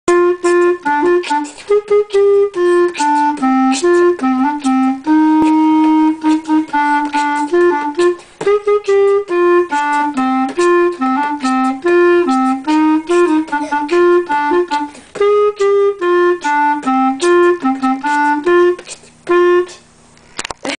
Kefa and Joel Music Extravaganza 121308
Kefa smokin the keys on the clarinet and Joel beat boxin like its 1999!